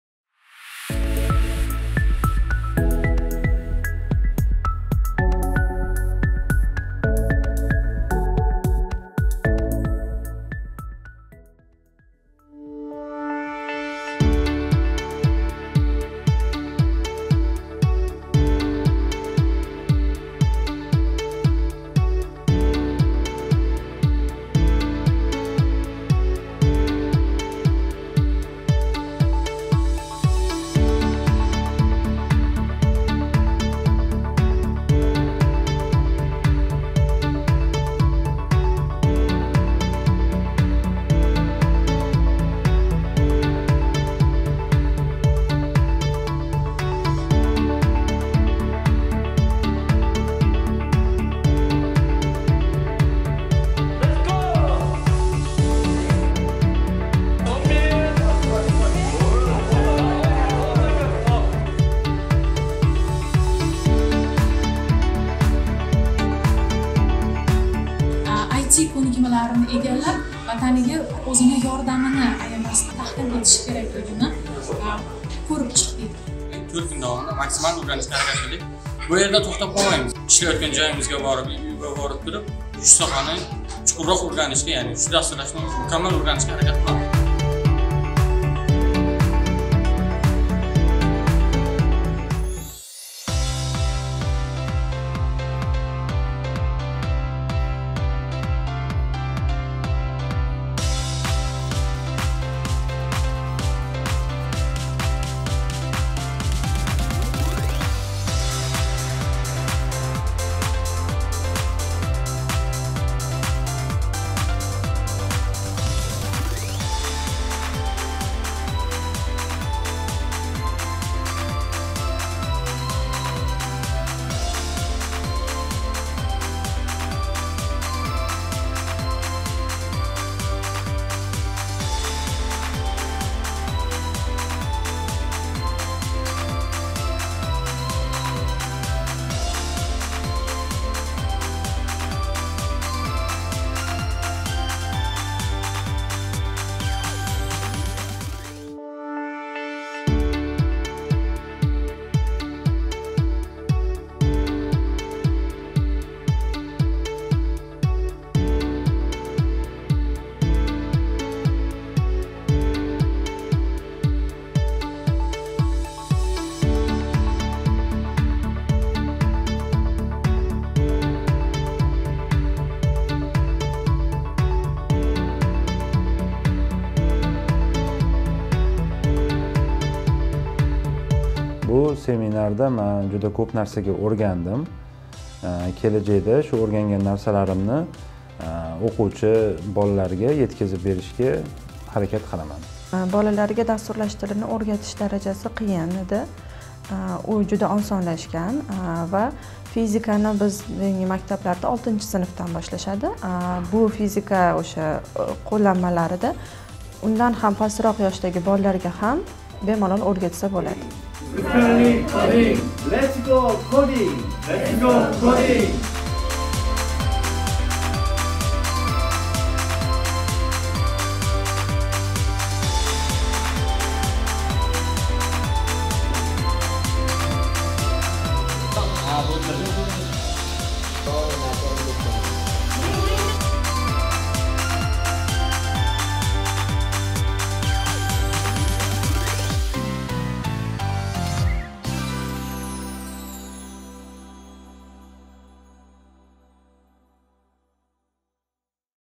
Uzbekistan Foreign Experts Group 2021
UEF(Uzbekistan Foreign Experts) guruhining 2021 yildagi faoliyatini tanishitradi.